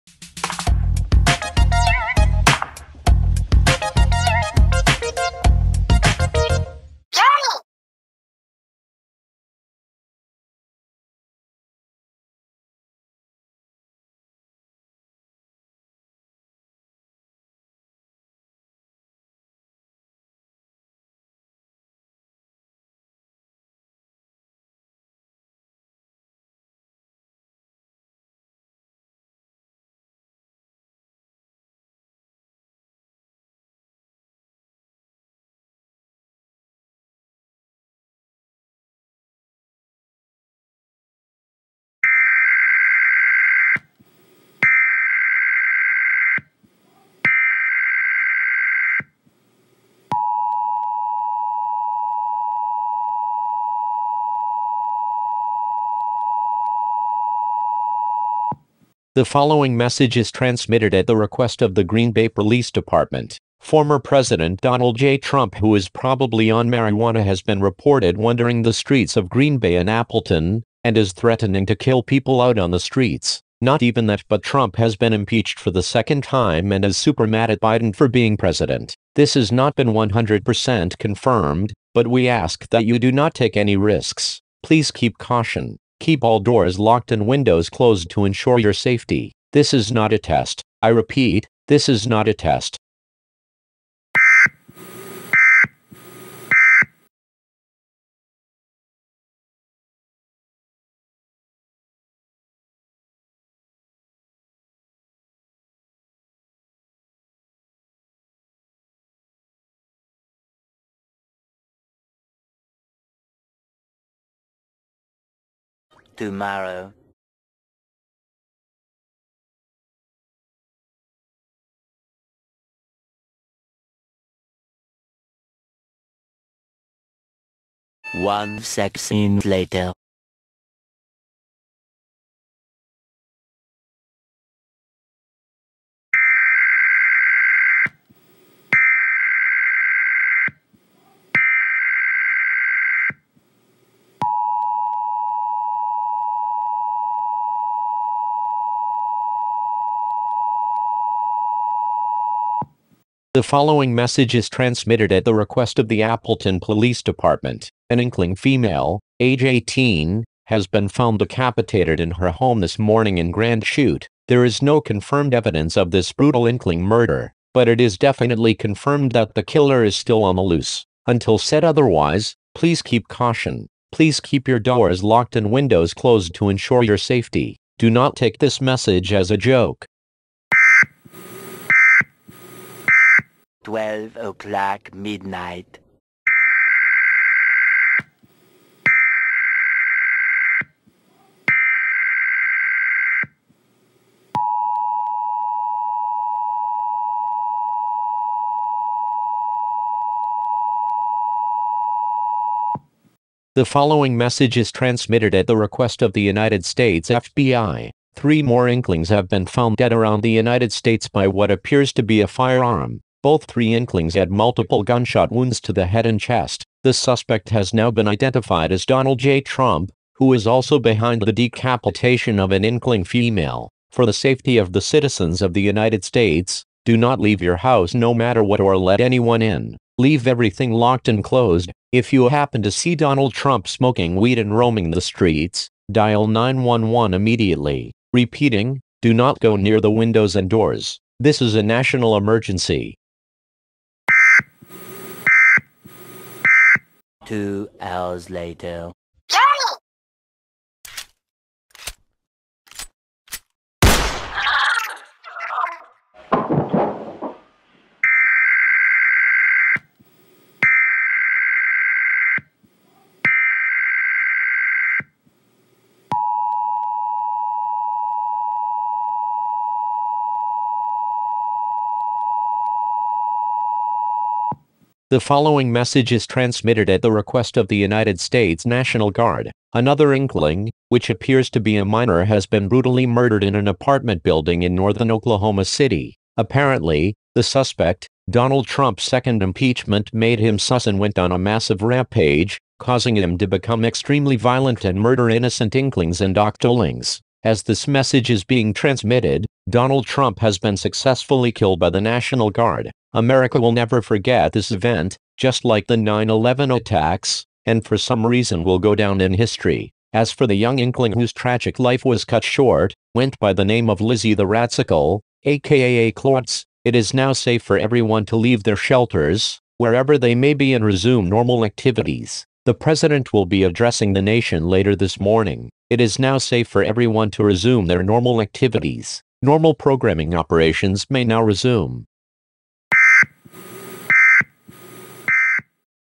EAS Scenario: Donald Trump Rampage (Part 2)
The final part of the EAS Scenario of Donald Trump is complete and will go down in history

EAS Alerts Relayed by me

Video uploaded by Meggy Spletzer